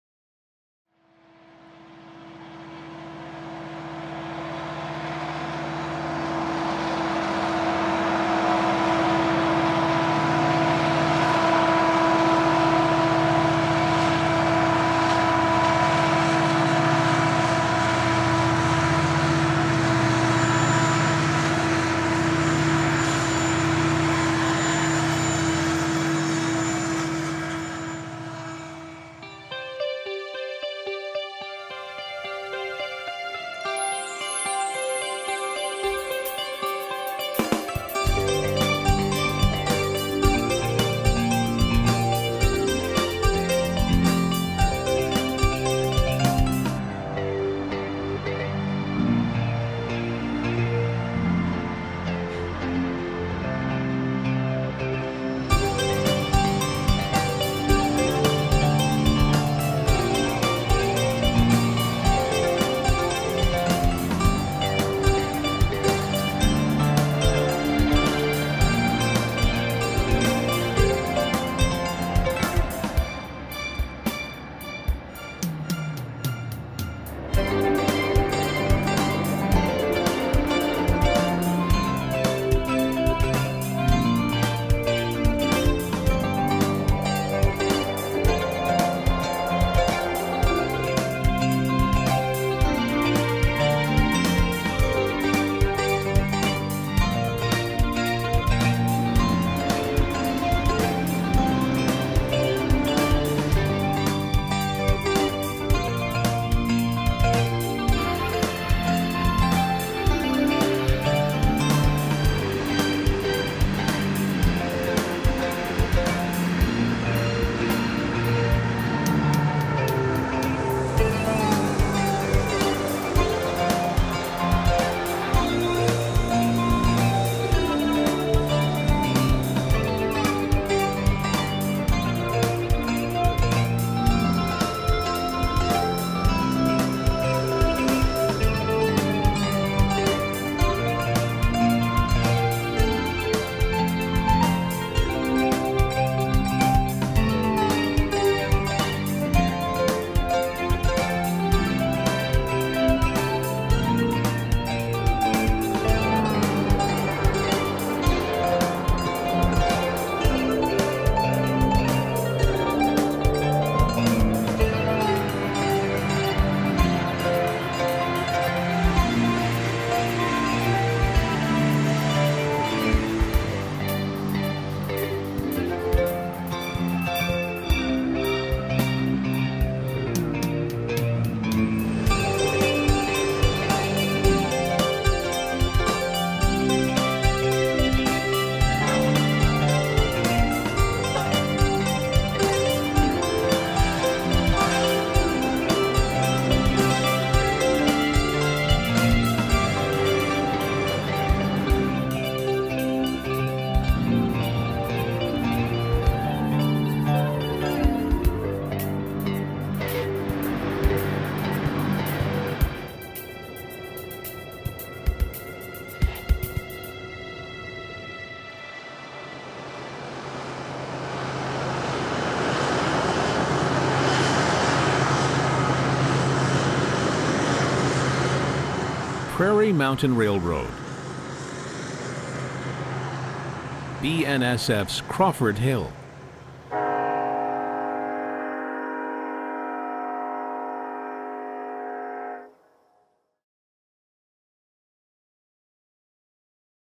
Crawford Hill Music Video 720p
Some highlight clips from this winter's upcoming feature release:
Prairie Mountain Railroad - BNSF's Crawford Hill.
4 hours! Footage was shot on several shooting trips from 2006 to 2012 and showcases a nice variety of power and action. We go from the mines all the way to a power plant, but the tour, of course is primarily about the famous 14 miles of mountain-like railroading found in the High Plains on Nebraska's Crawford Hill. With legal trackside access thanks to the Lund Family's Ponderosa Guest Ranch, this is one of America's greatest railfanning treasures!
The disks will include two complete tours of the hill, plus special features like a slide show and bonus clips from around the area, and a few surprises. Look for this show in the winter of 2013 / 14 from Iron Horse America - Video / Media.